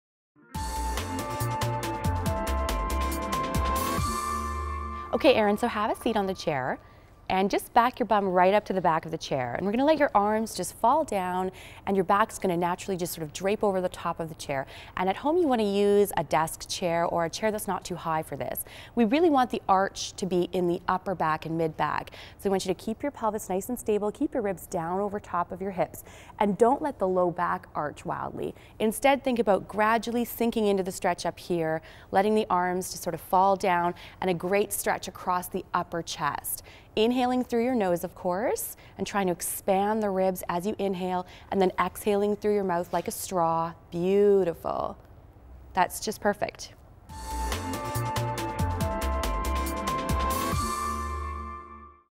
Postpartum exercise: Chest stretch on chair
Motherhood can be tough on your posture! Help relieve tension and restore alignment with this chest stretch. Dara Bergeron, founder of Belly Bootcamp, leads you through this move that you should practice for 30 to 60 seconds daily.